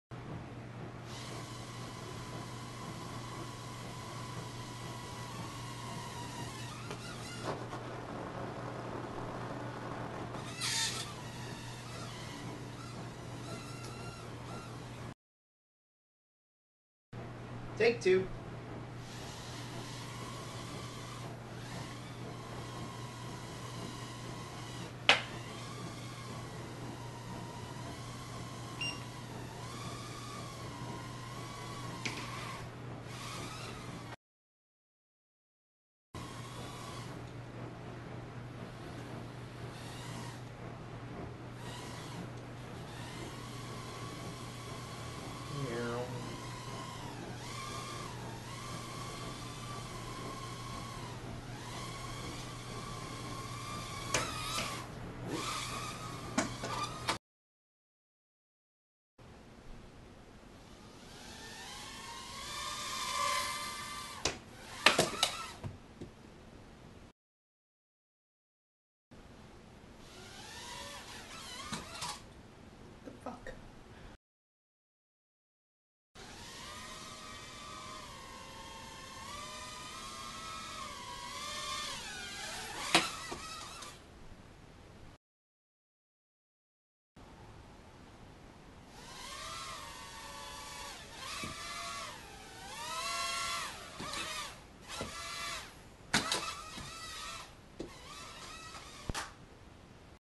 First attempt at some awful droning
Crashing the Propel Quark Micro Drone 2.4 Ghz Indoor/Outdoor Quad Rotor Helicopter all over my apartment